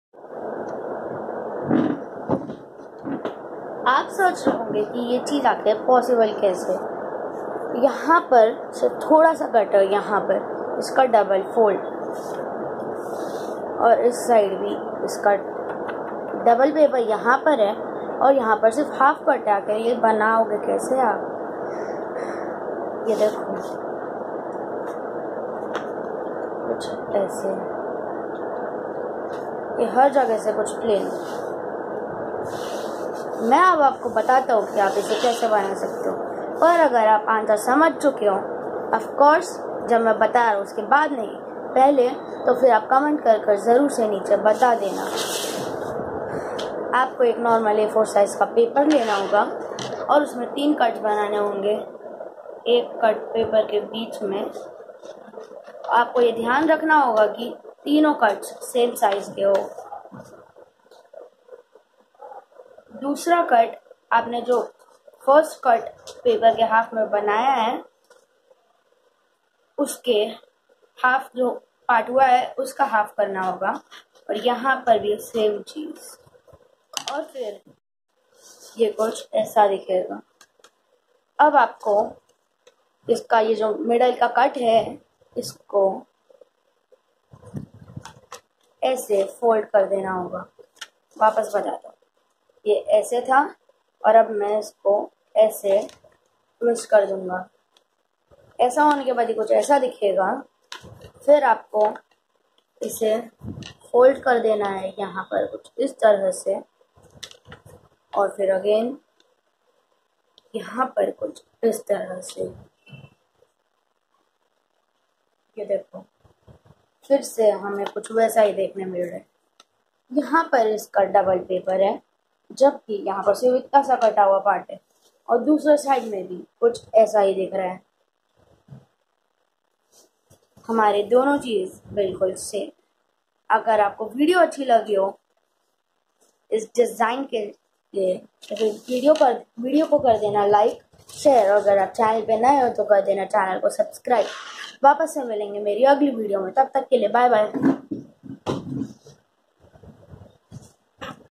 Paper Folding Puzzle Its Awesome
Nice paper folding puzzle to have fun with your friends and family, do try this once.